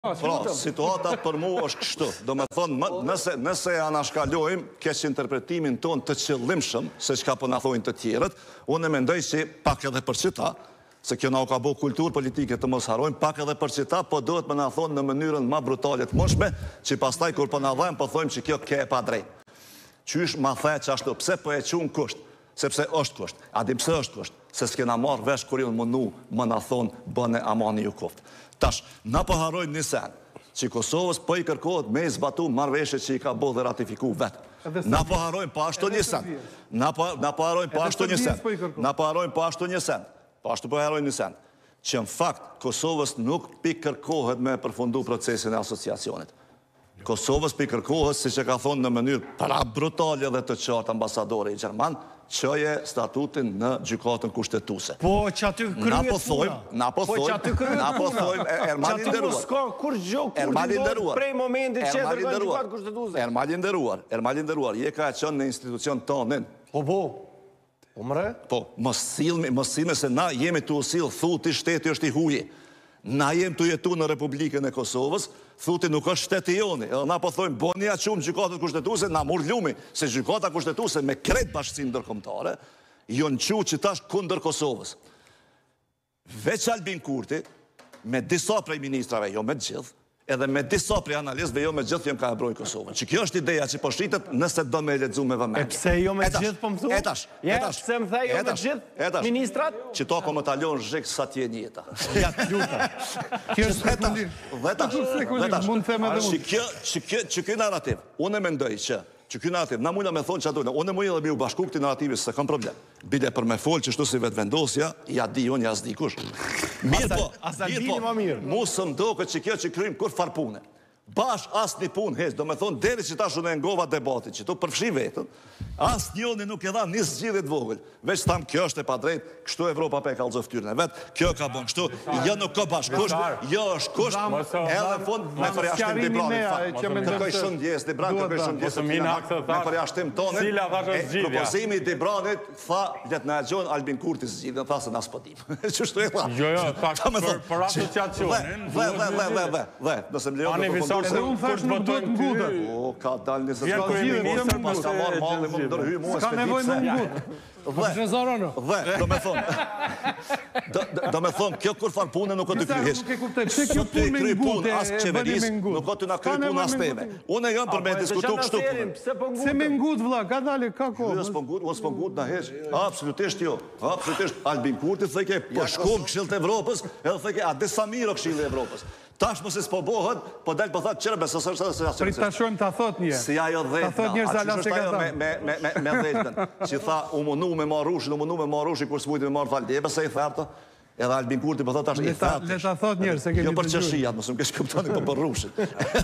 Ymeri: Ja di unë me fol si Vetëvendosja ja askush në këtë studio nuk din
“Kom me ta lanë zhig sa t’është jeta”, Visar Ymeri: Ja di unë me fol si Vetëvendosja ja askush në këtë studio nuk din